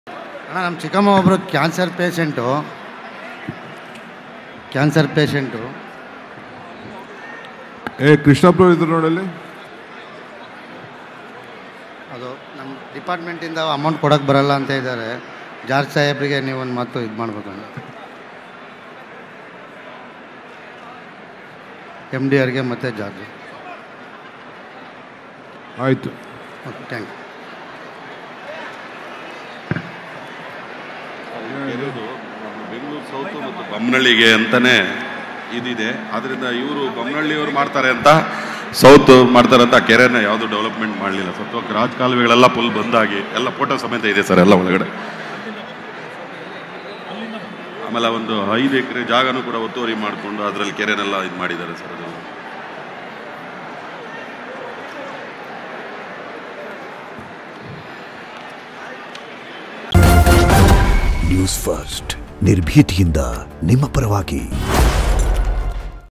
DCM DK Shivakumar : ಅಣ್ಣ ನಮ್ಮ ಚಿಕ್ಕಮ್ಮ Cancer Patient​ ಹೆಲ್ಪ್​ ಮಾಡಿ.. | Janaspandana | Newsfirst
DCM DK Shivakumar : ಅಣ್ಣ ನಮ್ಮ ಚಿಕ್ಕಮ್ಮ Cancer Patient​ ಹೆಲ್ಪ್​ ಮಾಡಿ.. | Janaspandana | @newsfirstkannada